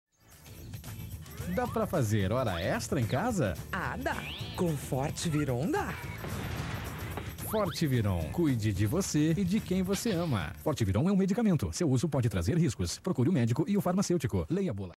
Forteviron | Comercial (2011)
Comercial fofo e engraçado do estimulante sexual "Forteviron". Confira!